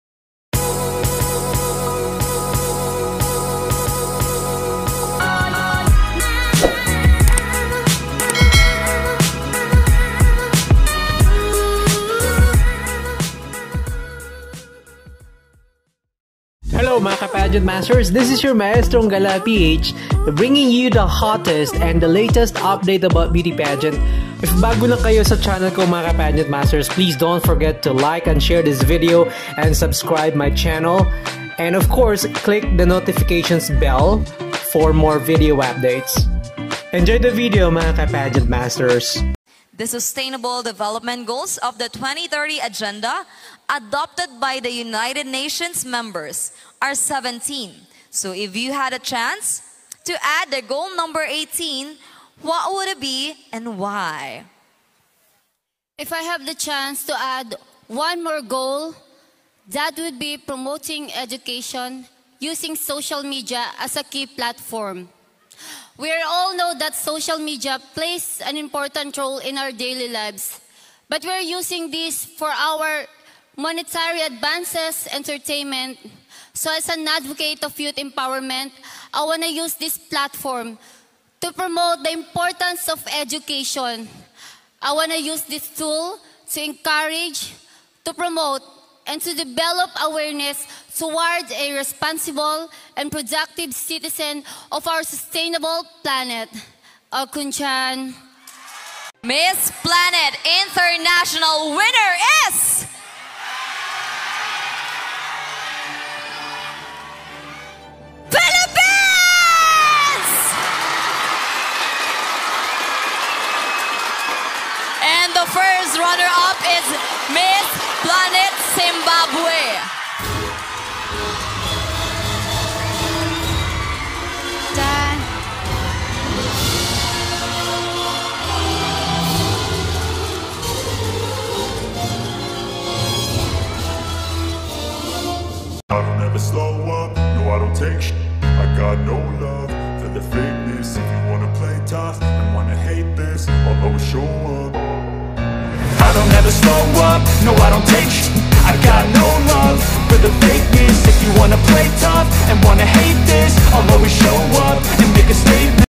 Winning Answer of Miss Planet International 2023- Maria Luisa Varela/Philippines. Congratulations!
Winning Answer of Miss Planet International 2023 from the Philippines. Congratulations!

Final Question and Answer Portion
Miss Planet International 2023